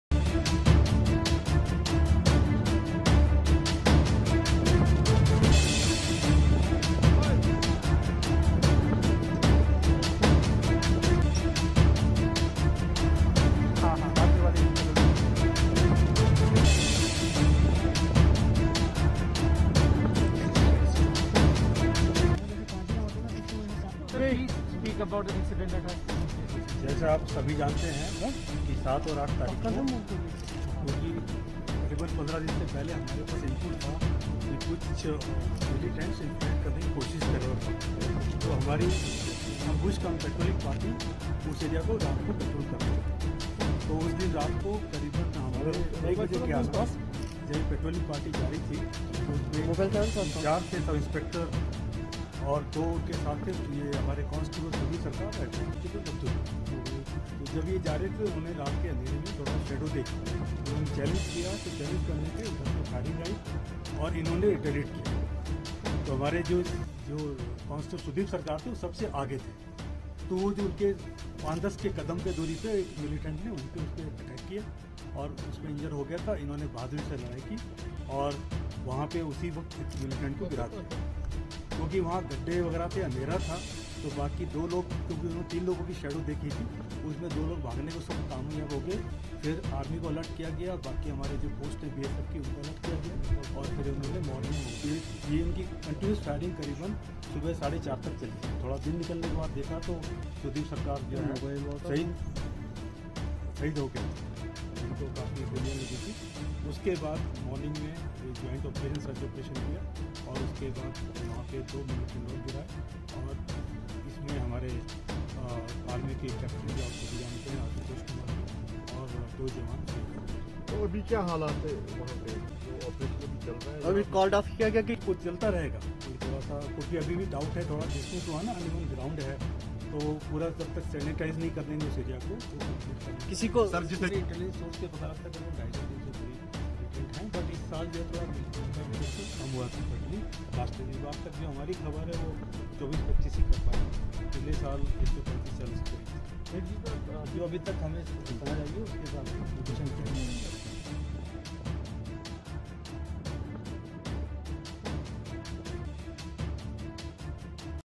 Constable Sudip Kumar : देश सेवा में हुए शहीद BSF |Jammu Kashmir कुपवाड़ा में घुसपैठ की कोशिश नाकाम
Constable Sudip Kumar : देश सेवा में हुए शहीद | BSF : कुपवाड़ा में घुसपैठ की कोशिश नाकाम , आतंकियों के खिलाफ ऑपरेशन में तीन जवान शहीद

जम्मू कश्मीर में भारतीय सेना लगातार घुसपैठ की कोशिश कर रहे आतंकियों का सफाया कर रही है . अब इसी कड़ी में 8 नवंबर यानी आज सवेरे बॉर्डर सिक्योरिटी फोर्स ( बीएसएफ ) ने बड़ी कामयाबी हासिल करते हुए माछिल सेक्टर में घुसपैठ की कोशिश को नाकाम कर दिया और एक आतंकवादी को मार गिराया . बीएसएफ ने मारे गए आतंकी के शेष समूह को भी पीछे धकेल दिया . हालांकि इस मुठभेड़ में बीएसएफ कांस्टेबल सुदीप कुमार को भी प्राणों का बलिदान देना पड़ा .

जय हिंद 🇮🇳🇮🇳🇮🇳🇮🇳🇮🇳🇮🇳🇮🇳🇮🇳